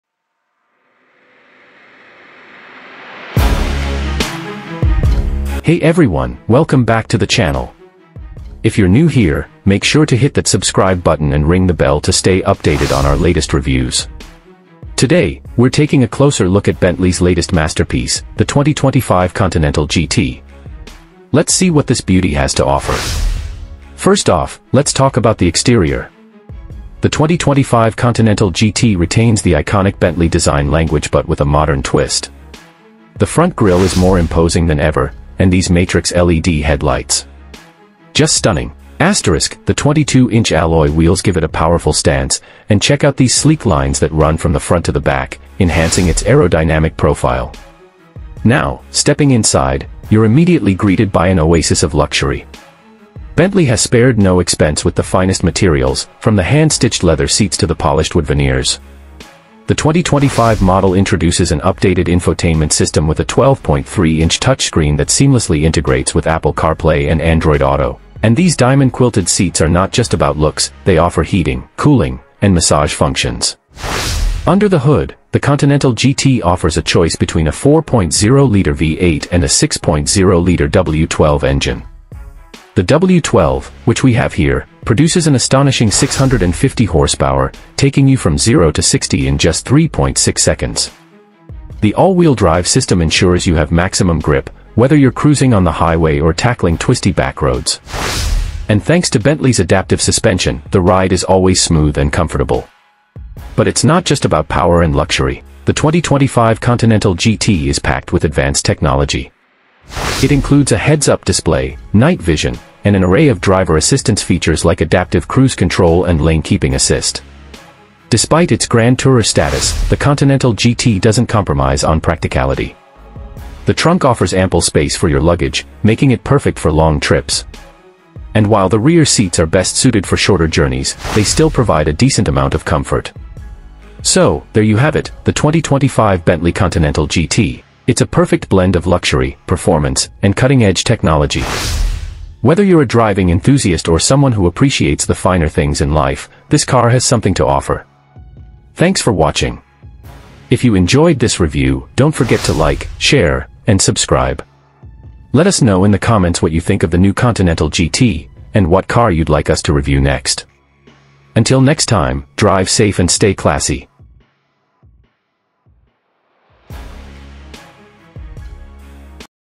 Luxury Redefined - Introducing the 2025 Bentley Continental GT"
Description
The 2025 Bentley Continental GT epitomizes luxury and performance with a blend of timeless elegance and cutting-edge technology. Its sleek, aerodynamic design hints at its formidable power under the hood, featuring advanced engineering that promises exhilarating performance on both city streets and open highways. Inside, opulence meets innovation with handcrafted materials and state-of-the-art amenities, creating an unparalleled driving experience. The 2025 model continues Bentley's legacy of craftsmanship and attention to detail, offering drivers and passengers alike a sanctuary of comfort and refinement. With refined lines, powerful engines, and luxurious interiors, the 2025 Bentley Continental GT sets a new standard in grand touring excellence.

1.
2.
3.
4.
5.
6.
7.
8.
9.
10.